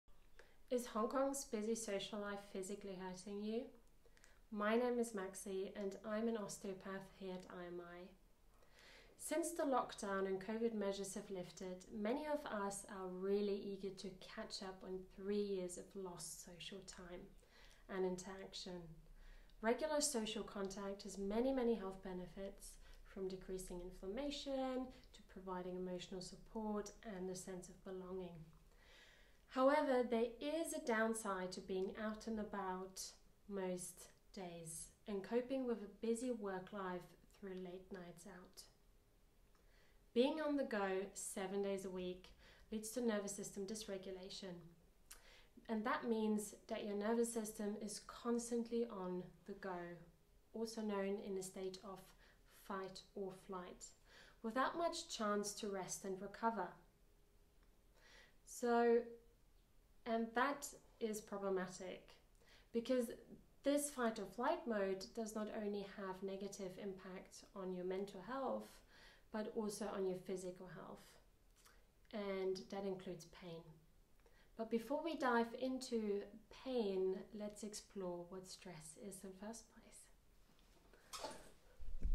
1. Post-pandemic "Fight or Flight" reactions? Osteopath Maxi Schönteich explains.
Since the lifting of Covid restrictions, many of us have hit the ground running when it comes to reconnecting socially.

Though it’s exciting to taste the freedom we’ve missed for a long time, our bodies can interpret this sudden shift as stress, moving us into fight or flight, chronic stress and pain.

In these videos on this playlist, osteopath and pain expert Maxi Schönteich shares a technique to switch your body from fight and flight into rest and digest, for more relaxation.

Research has shown that another powerful way to soothe your stress response is safe, therapeutic touch. If you’re suffering with either stress or pain, Maxi can help.